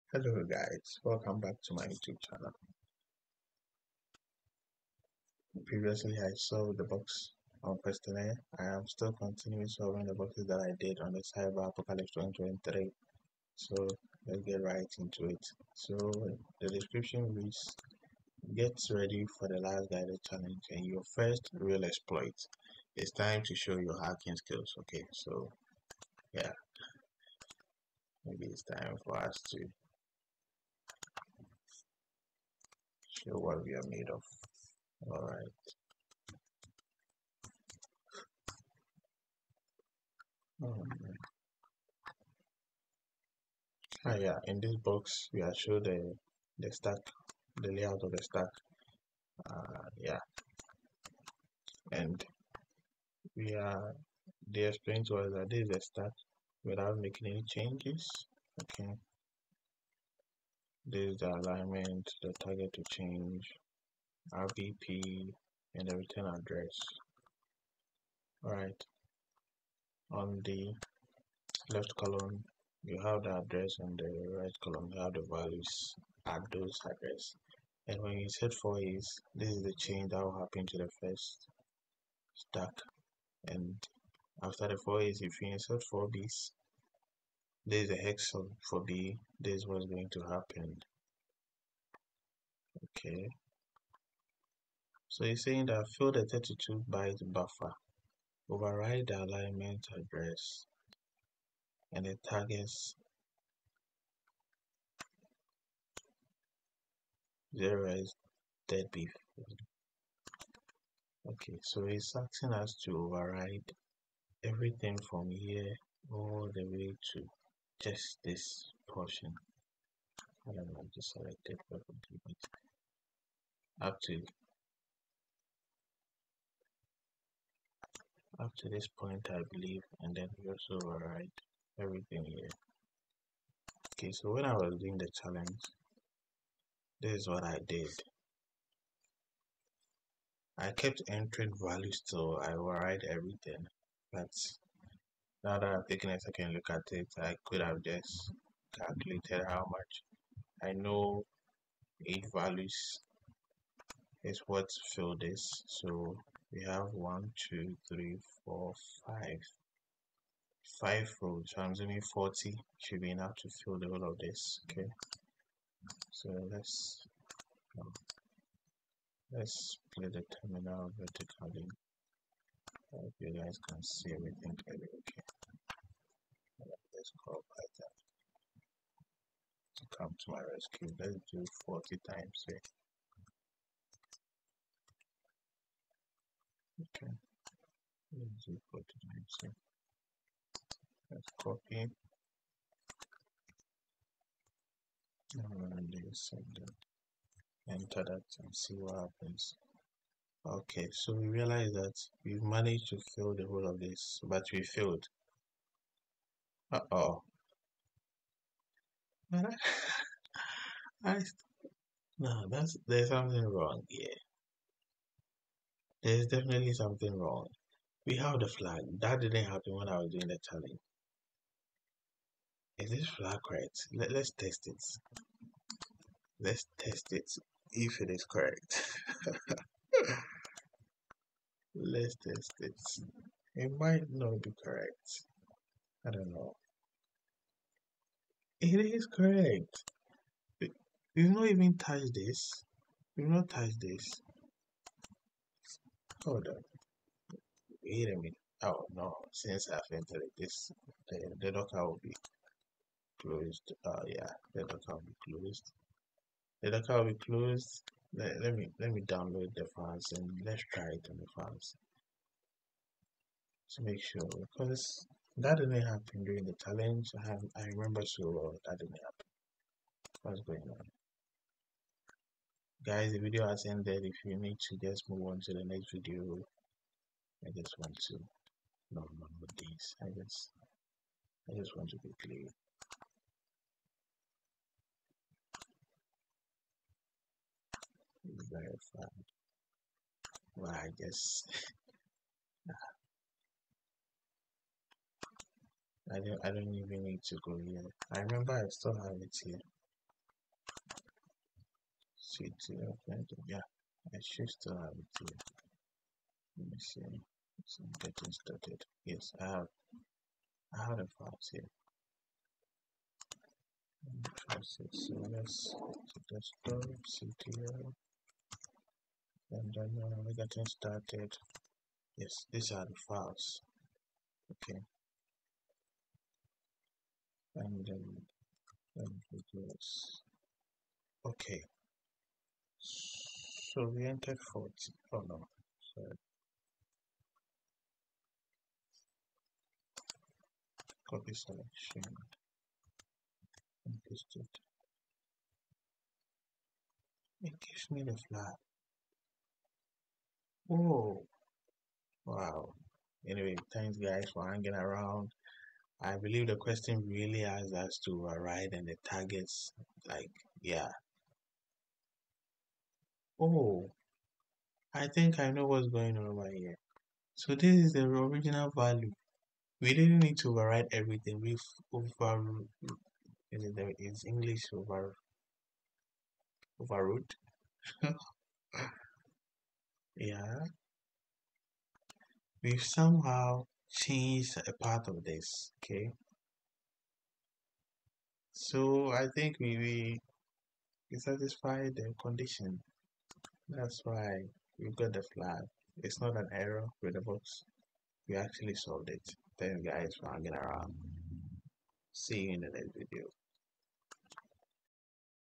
HTB CTF 2023 - Getting Started (pwn)
In this video, I demonstrate how I completed the "Getting Started" challenge in HTB's Cyber Apocalypse - The Cursed Mission CTF.